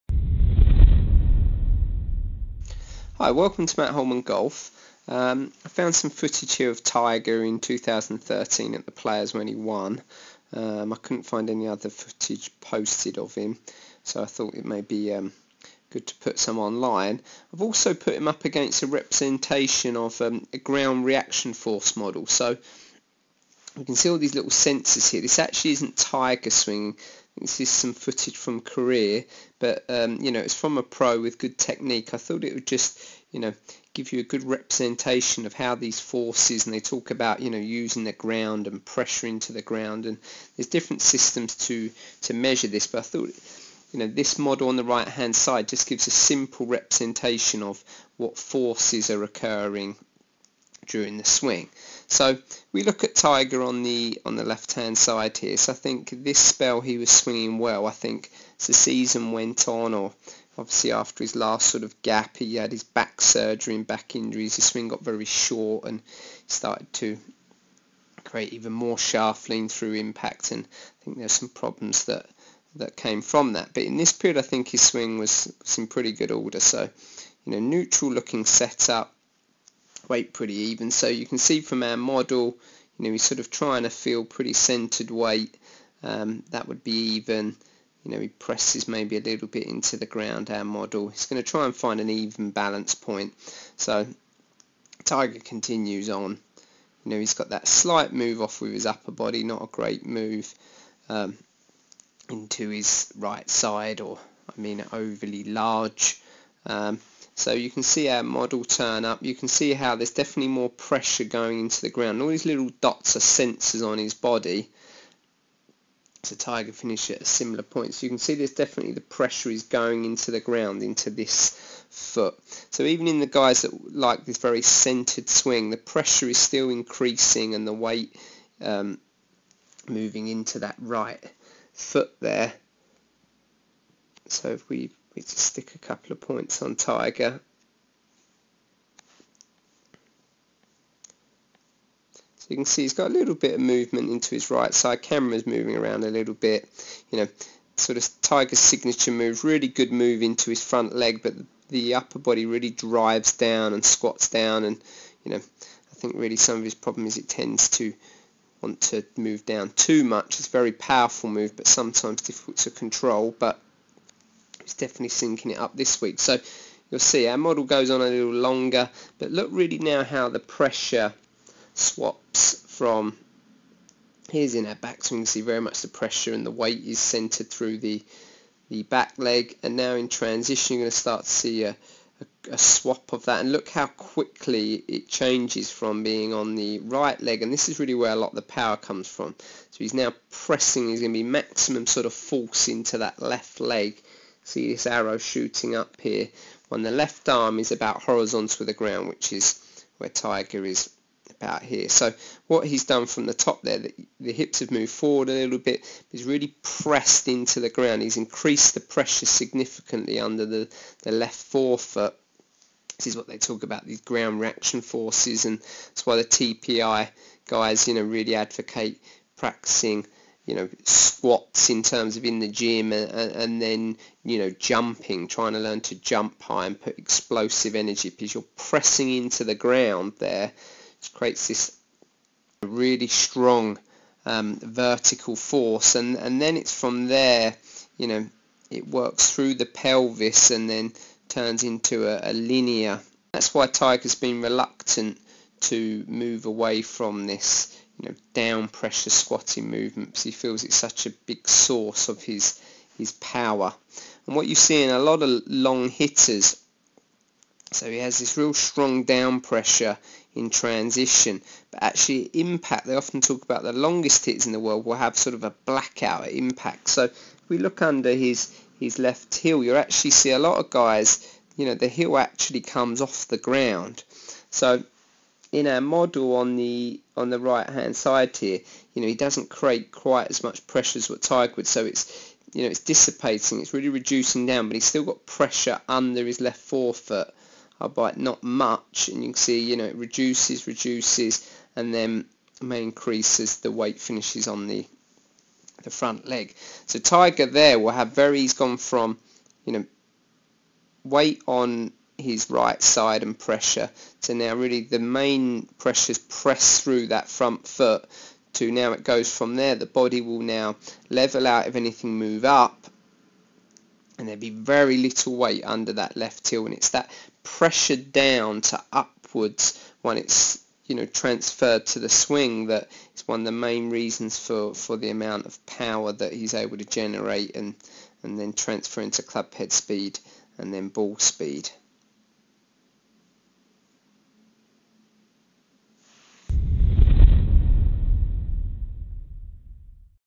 Tiger Woods 2013 Iron Swing: Ground Reaction Forces
Slow motion footage from Tigers 2013 Players Championship victory combined with a ground reaction force model. Analysis my Matt Holman P.G.A Advanced Professional.